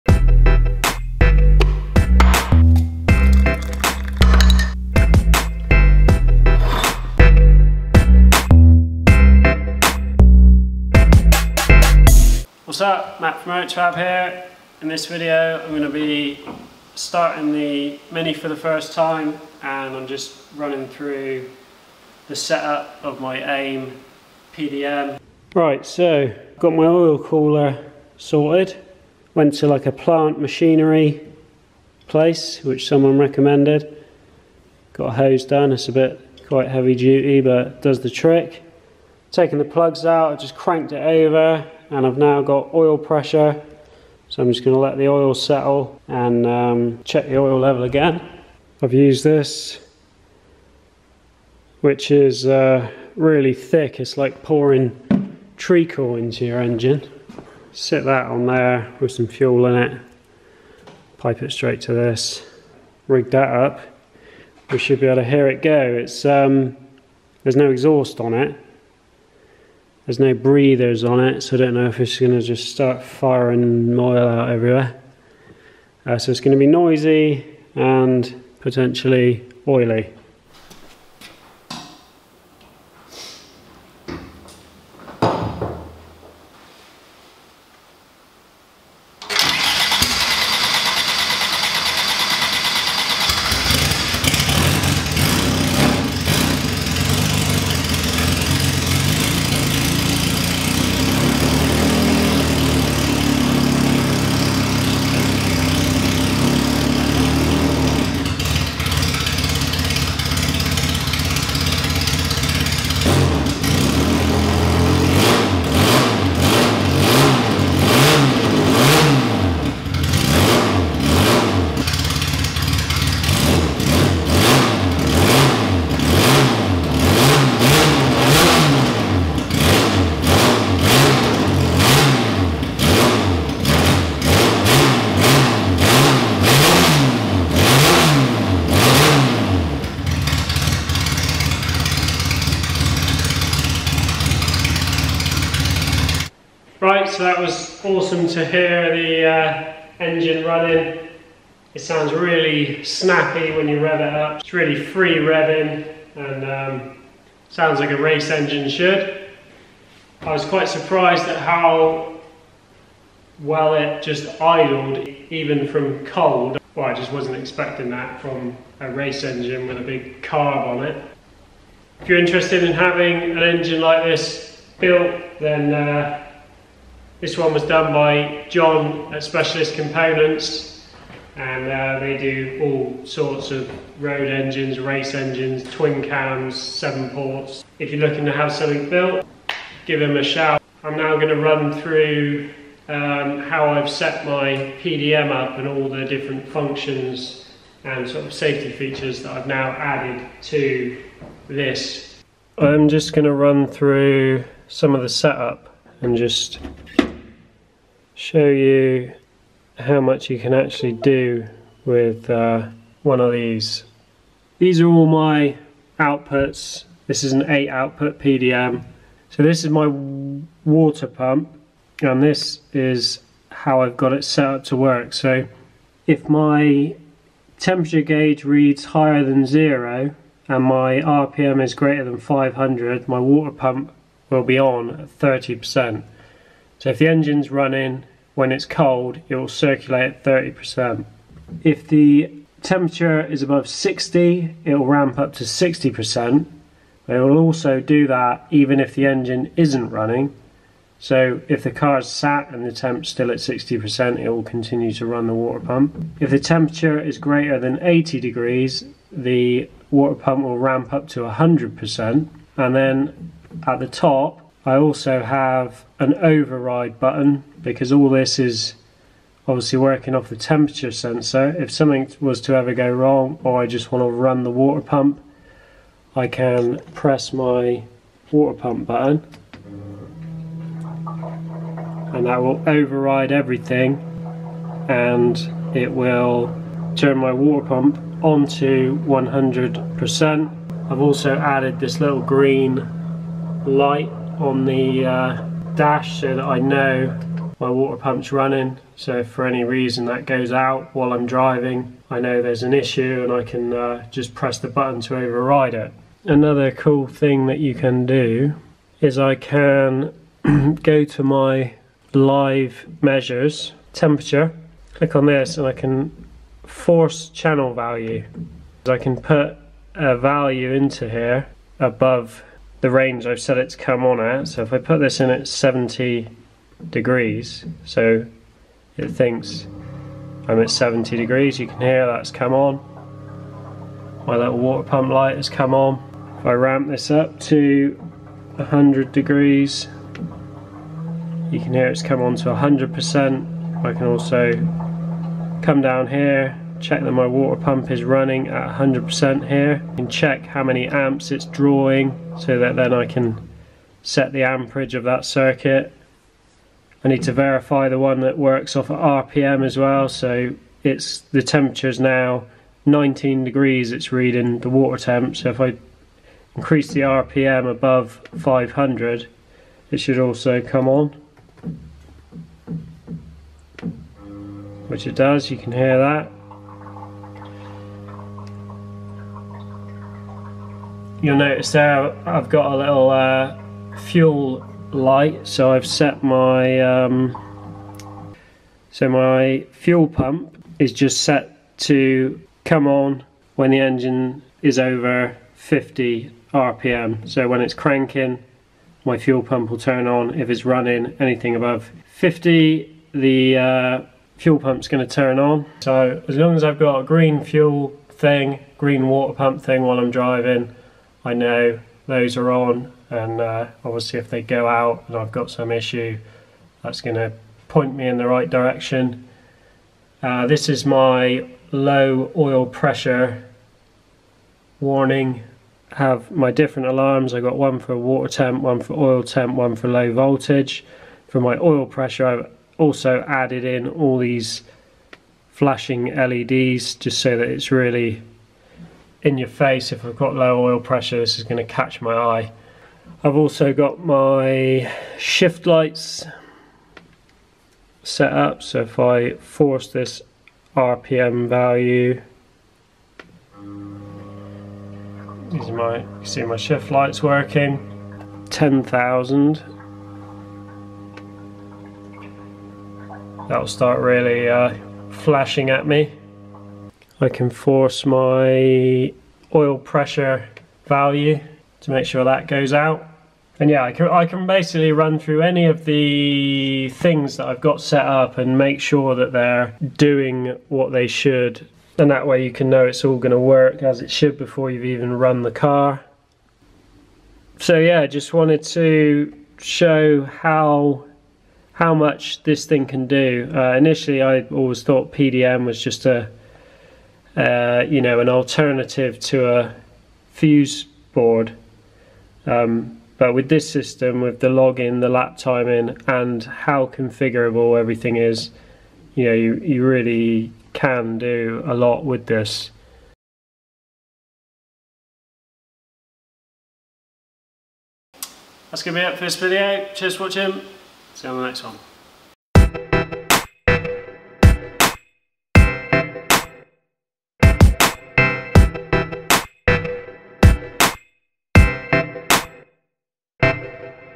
Classic Mini Race Car First Start + Aim Pdm Setup
Today I get to hear my Specialist Components 1293cc race engine for the first time. I'm also setting up my Aim pdm system.

Urchfab Front Subframe Kit
Urchfab Mini Shifter Mount
Spray Gun Holder
Mini Toe Board Kit
Mini Toe Board Light Weight